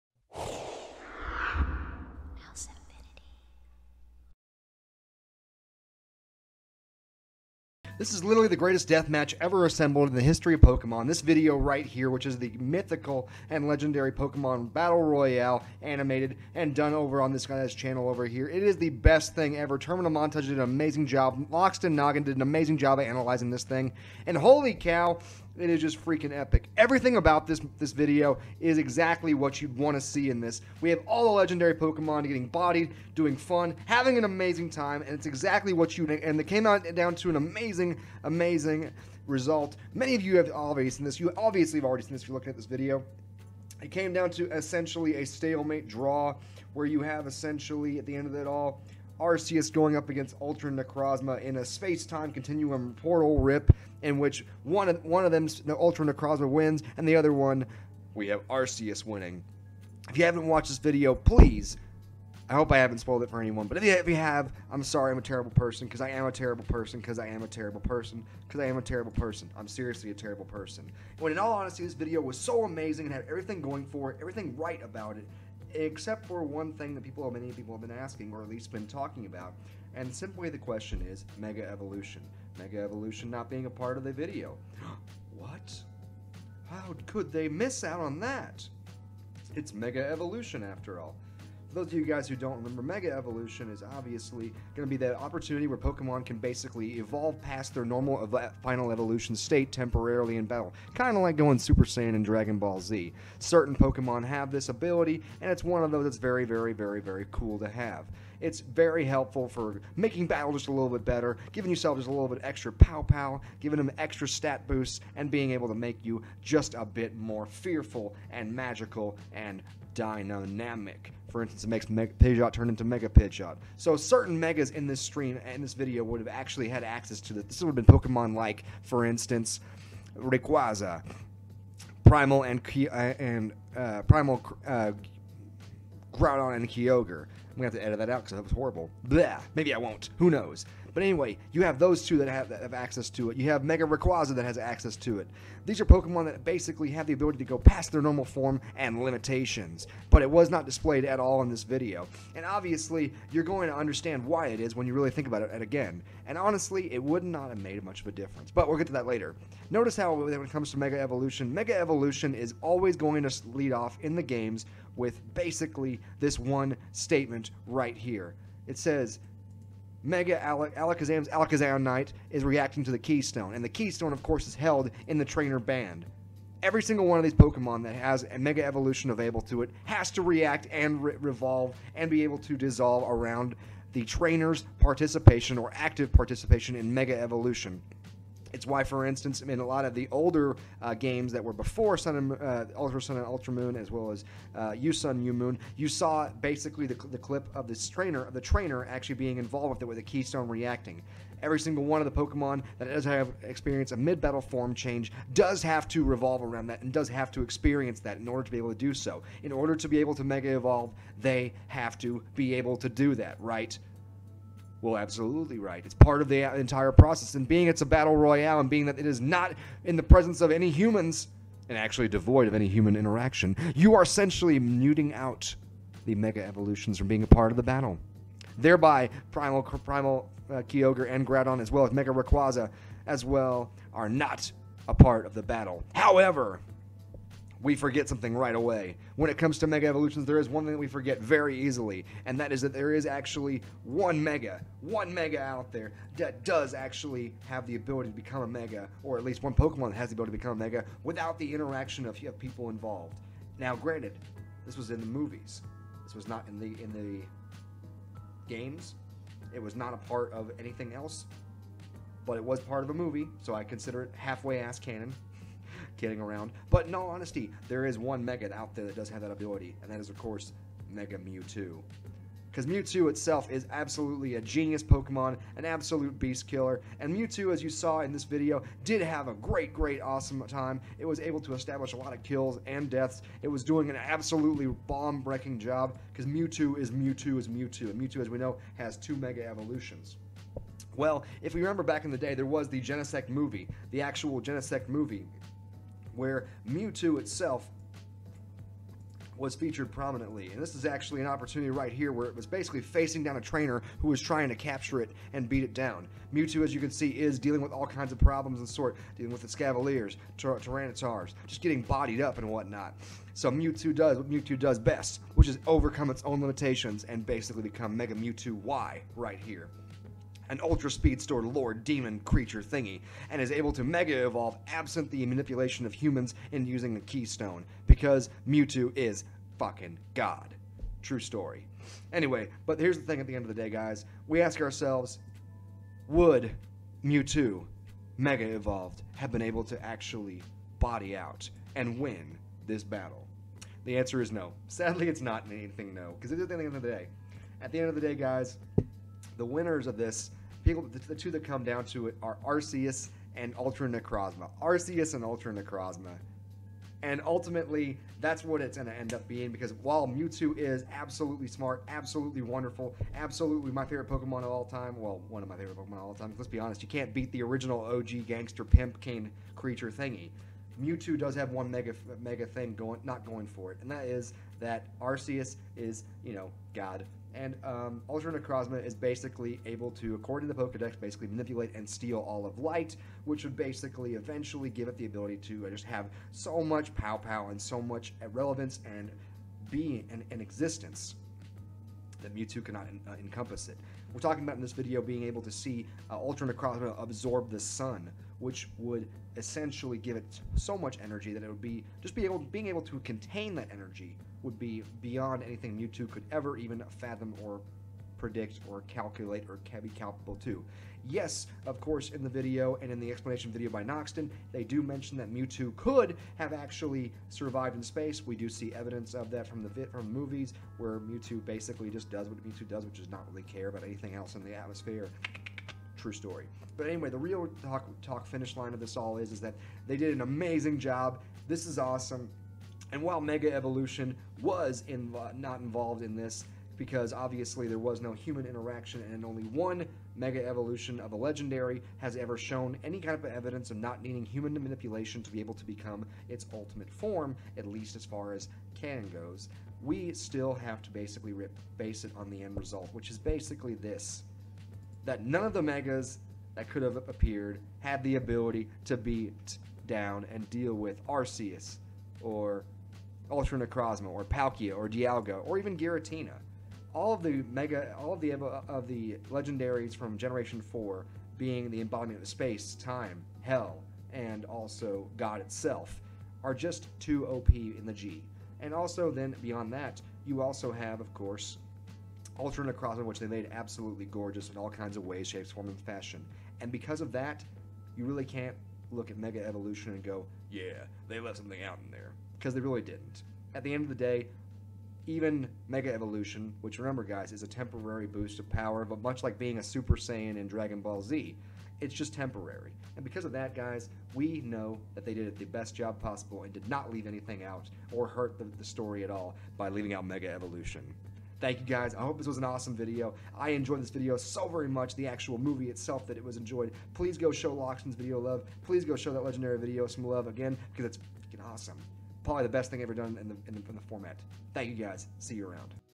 No Mega Evolution?? In Legendary & Mythical Pokemon Battle Royale: explanation
Terminal montage has come back and made the best video of 2019 for the pokemon commmuntiy there it said it! but there was one small thing i and others were confused by. mega evolutions. they weren't present but why? this video explains why and also why it would not have mattered.